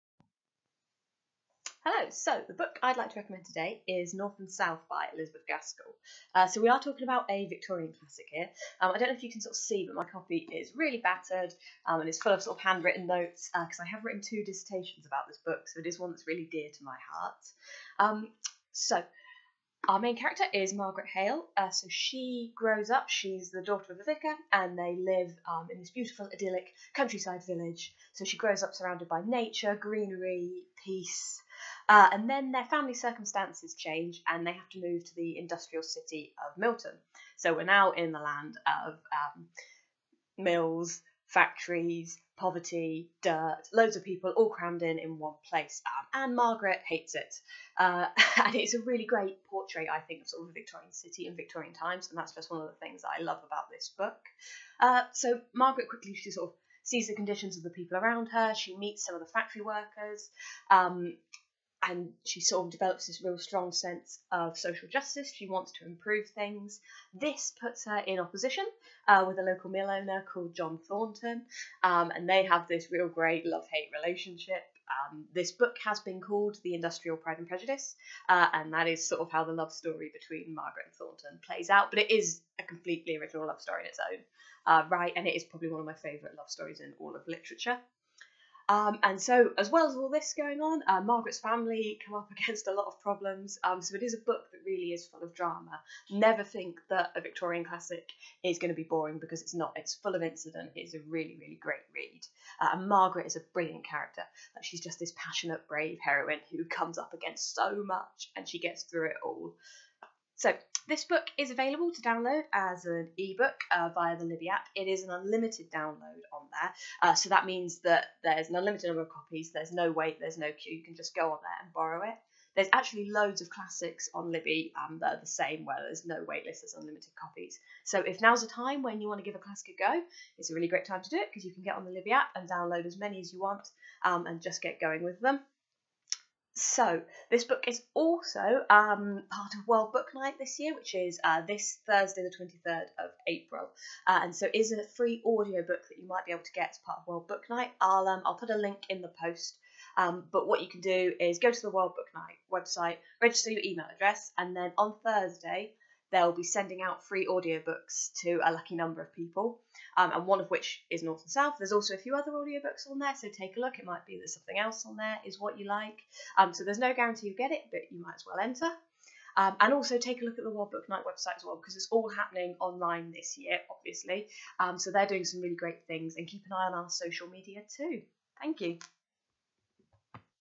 Book Recommendation: North and South
Chloe recommends North and South by Elizabeth Gaskell. Available from Devon Libraries as an unlimited download ebook via the Libby app. It is also one of four free audiobooks you can sign up for a chance to receive as part of World Book Night on Thurs 23rd April